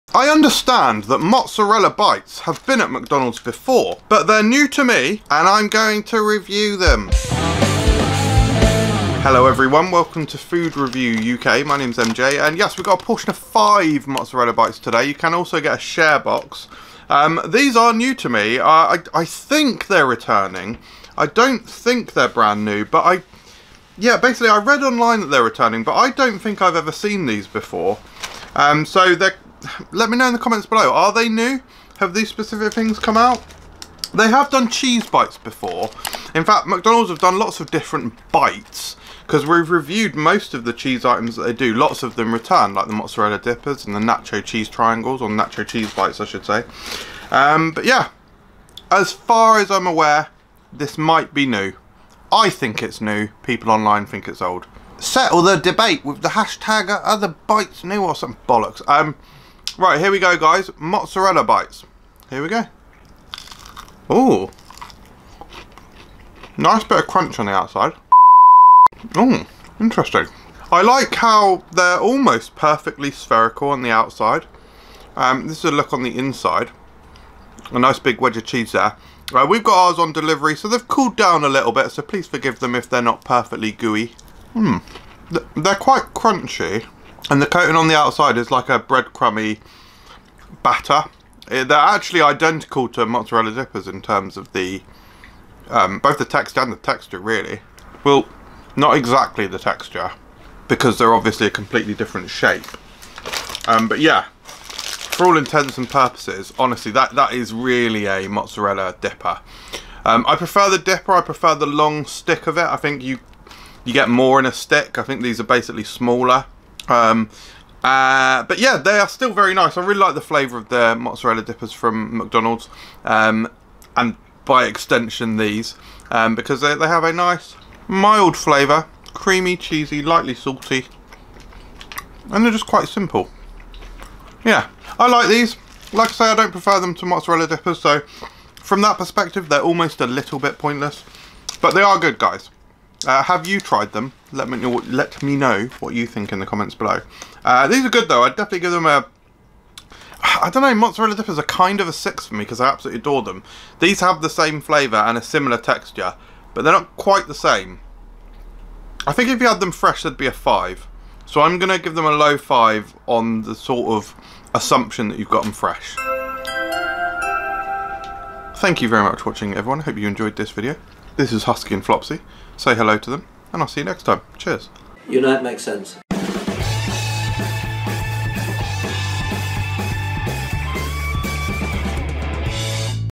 McDonald's Mozzarella Bites Review (basically Mozzie D's)
Mozzarella Bites are available from McDonald's. They're small globes of molten mozzarella in a breadcrumb coating. Very crispy. Available in 5 or a sharebox.

- use "FRUK20"
- use "foodreviewuk"